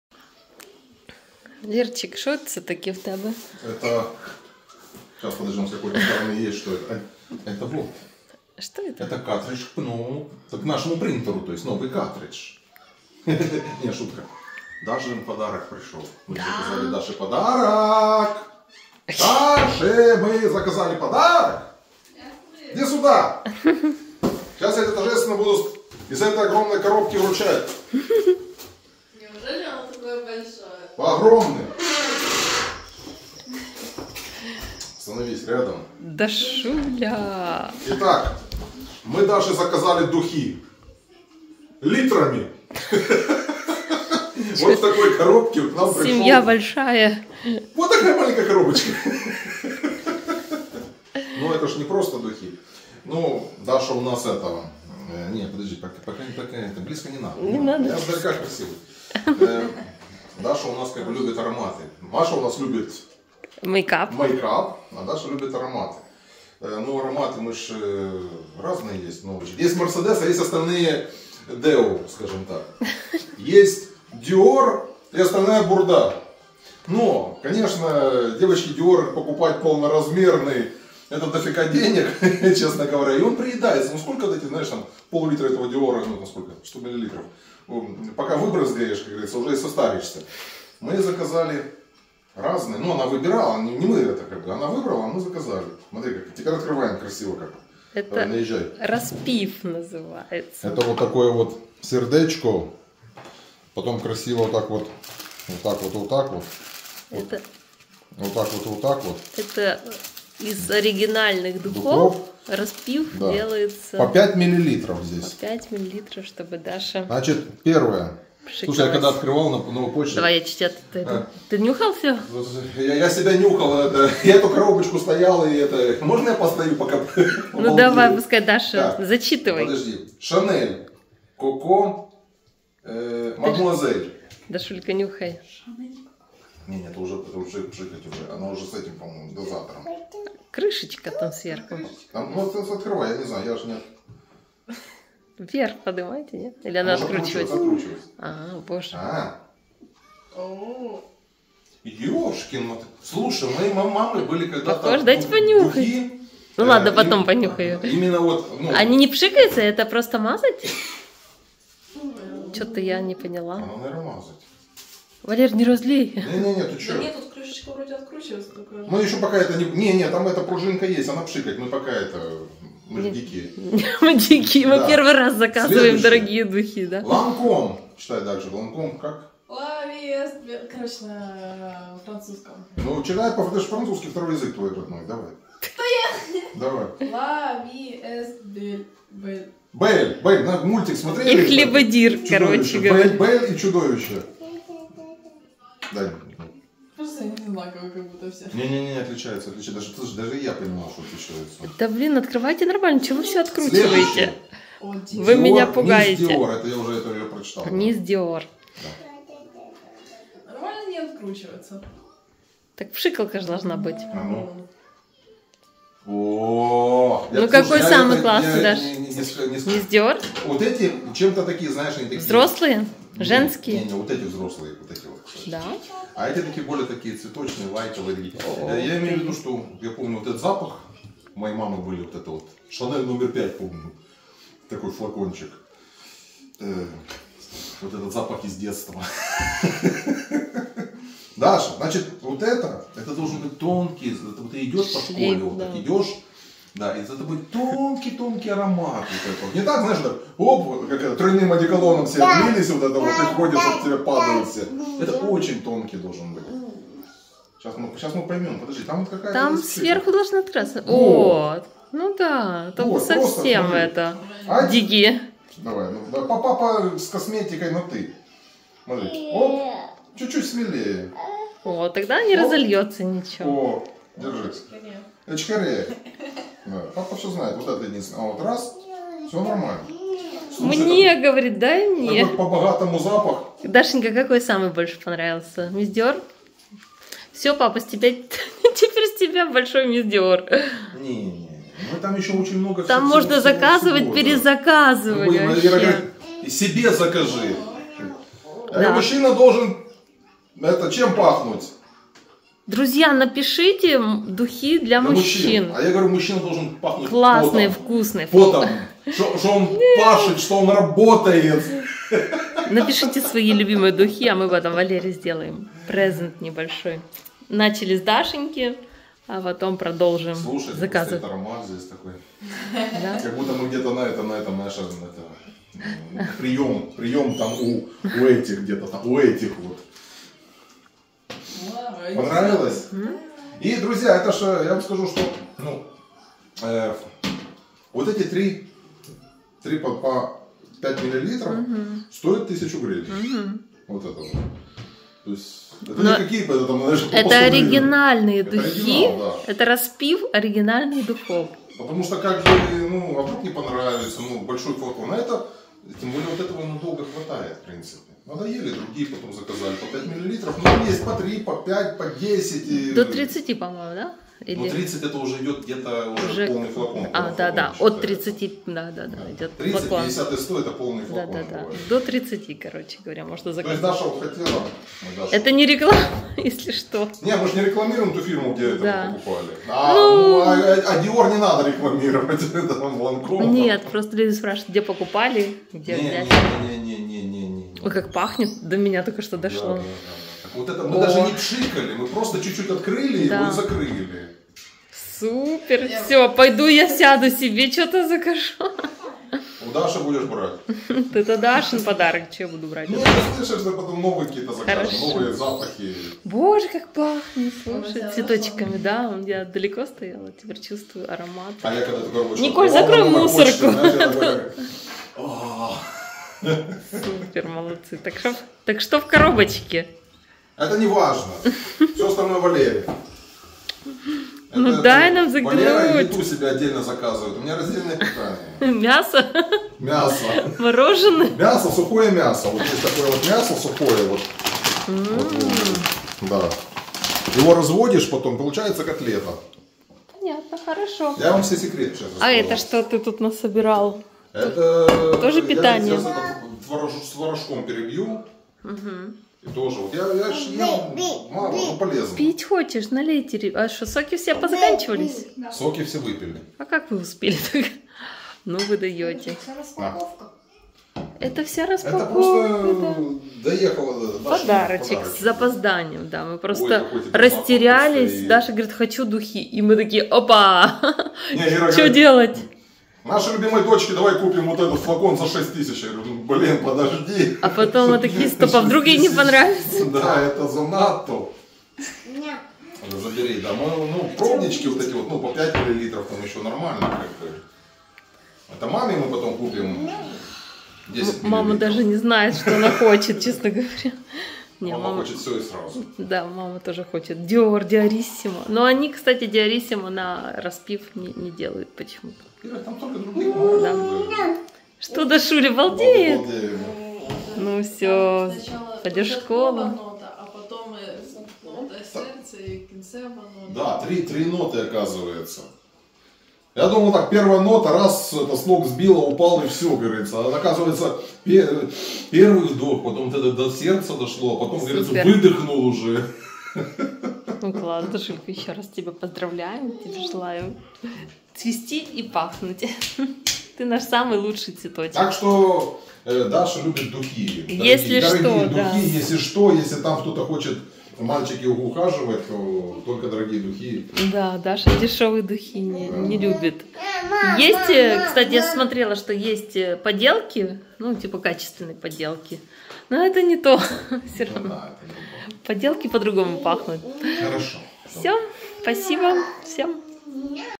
Подарок для Даши на ДР 🎁 Неизданное. Жизнь многодетной семьи из Украины
Жизнь многодетной семьи из Украины! Life of a large family from Ukraine!